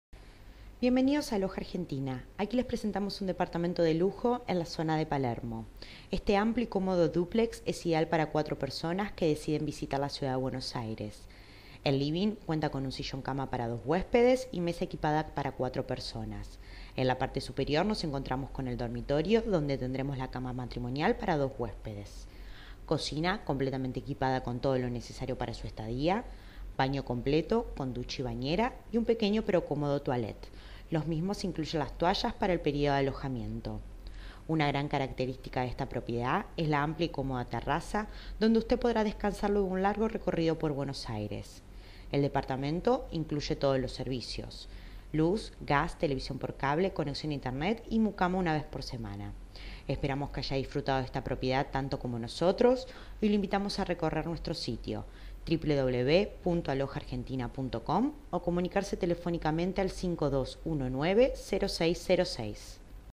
Furnished apartments Buenos Aires
Apartment Buenos Aires Palermo
A beautiful, luminous duplex for rent in Palermo.

The magnificent duplex with terrace located in brand new building. It is ideal for a couple who is enjoying its vacation or a business traveler since it has a double bed and a spacious room in the living. The living room has a dining area and are always full of light thanks to a sliding glass door that leads to the big terrace where you will find tables and chairs: the ideal place for rested, sunbathing, having breakfast, reading, and all you want to do when you are on holidays. It has a fully equipped kitchen with oven, perfect for preparing any kind of meals.

The bathroom is ensuit with bathtub. It is important to mention that in the first level you have a toilette, very practical when you have guests. In the living room, there is a sofa bed for one person, so this could be an alternative for those who are looking for a three -person apartment: a couple with a child or a friend.

This is a tasteful duplex with modern design

Area: Palermo Viejo is a cultural zone of Buenos Aires. But you can also find Entertainment due to the fact that you have many bars, pubs, restaurants offering different types of foods, such as Mexican, Japanese, etc. you will also find a trendy area in clothes and handmade objects, made by young people. Through Santa Fe Avenue, you will see many bus stops that lead the tourist to all capital neighbors, and the suburbs. There are also trains and subways that take you to Belgrano and downtown